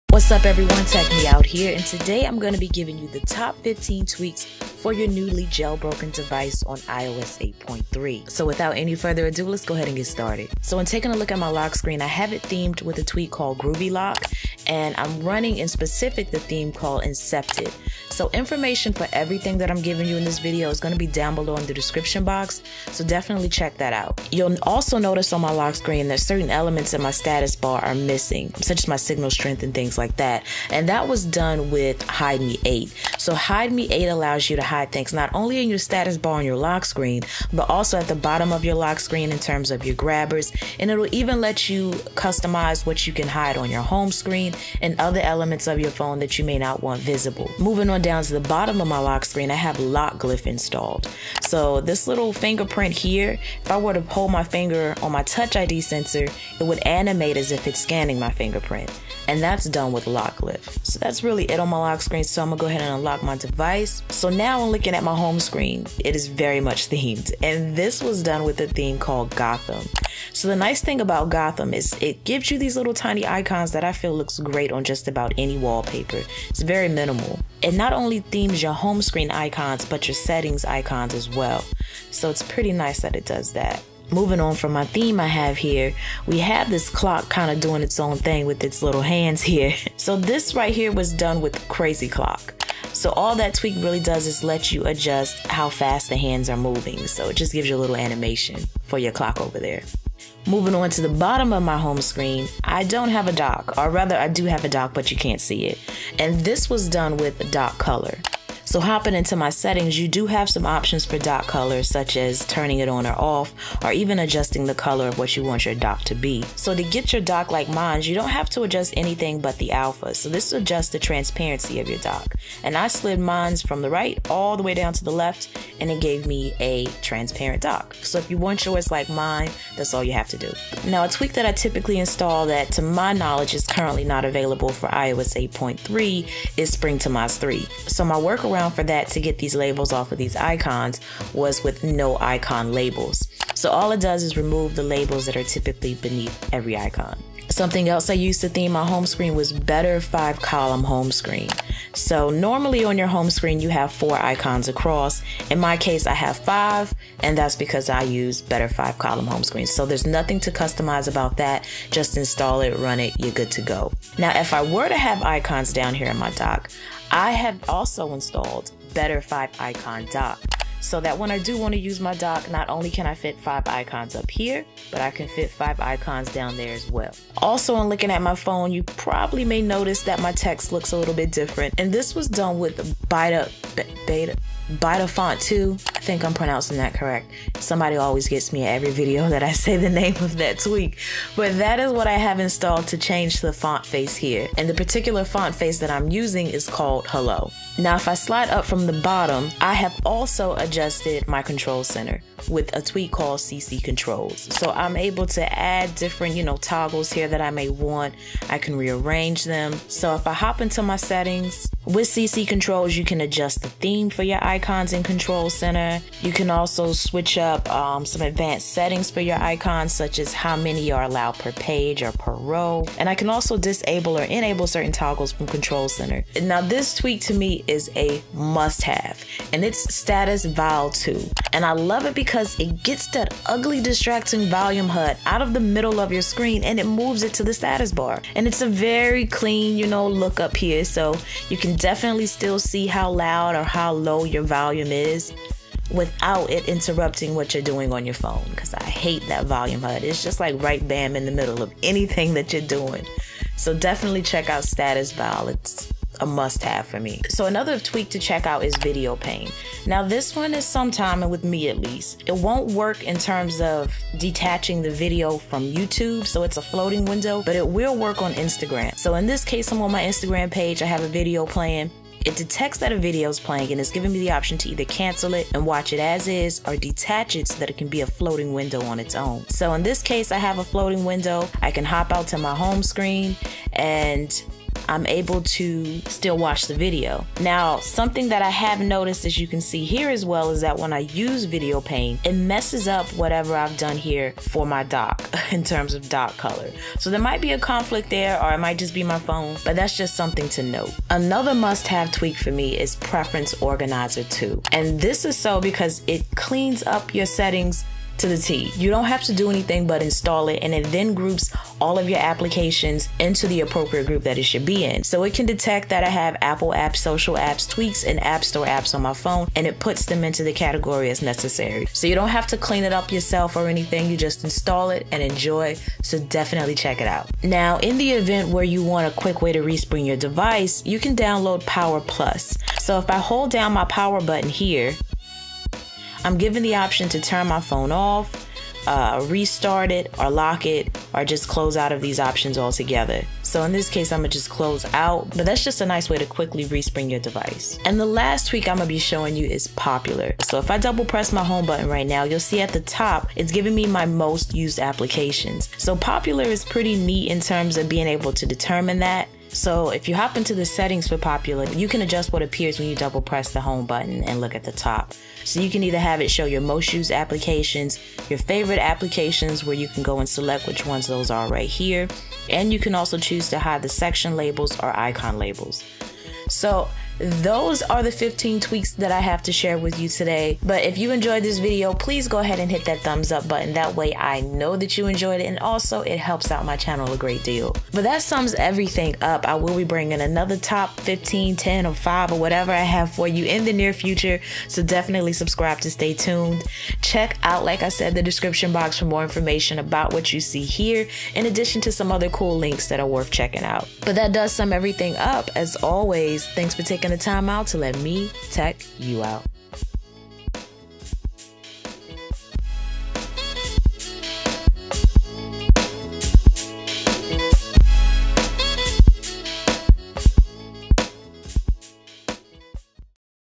Top 15 Tweaks for iPhone, iPad, and iPod
Top 15 tweaks for iOS 8.3 and iOS 8.4 or below.

M Y  N E W  W E B S I T E

T W E A K  L I S T

1) GroovyLock (Incepted theme) -  Free in Modymi
2) HideMe8 Lite -  Free in BigBoss
3) LockGlyph - Free in BigBoss
4) CrazyClock - Free in BigBoss
5) NoIconLables - Free in BigBoss
8) Byta Font 2 ("Hello" font face ) - Free in Modmyi
11) VideoPane - $1.99
 12) Preference Organizer 2 - Free in HackYouriPhone
 13 PowerPlus - Free in BigBoss
14) Popular - $.99 in BigBoss
15) Gotham Theme - Free in Modmyi